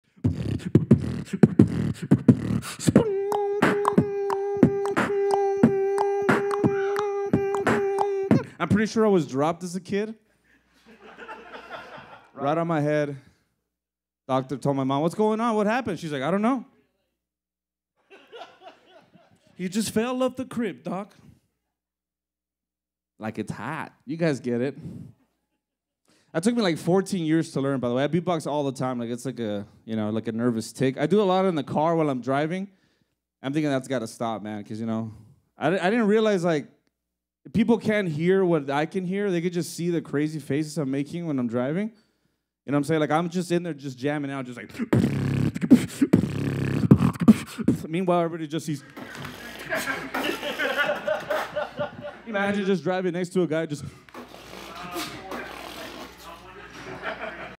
Beatboxing Comedian, Just Greg at the Open Mic
Enjoy this clip of El Paso comedian Just Greg at our open mic night at Mona Bar of Modern Art in October 2023.

Find our Event Calendar and join us at the open mic!

Follow/Support Richie D. Marrufo ►

BWOMS is available with the support of BorderSenses